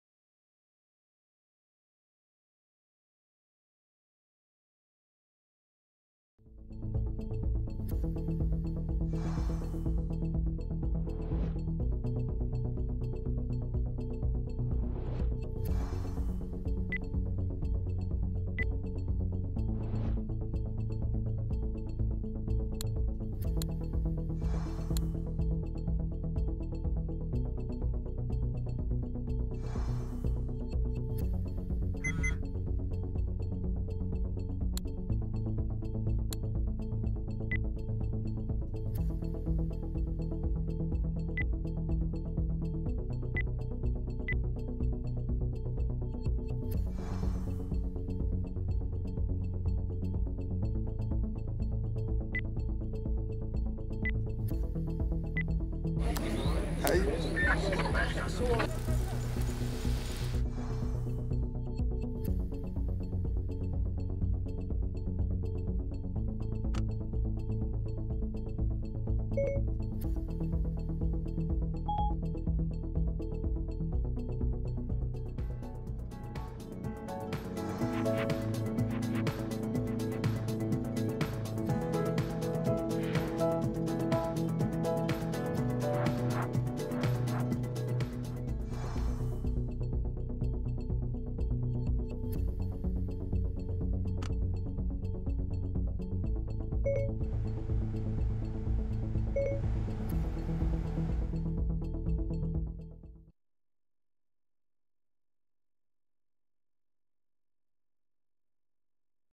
Honda Civic | Connected Driving Experience
Director: Gavin Gordon-Rogers
Lead Producer: David Lamb

The Honda Real View Test Drive enables you to explore the Honda Civic and see what it’s like to drive it in the real world. Find out why its clever features make it such a desirable car.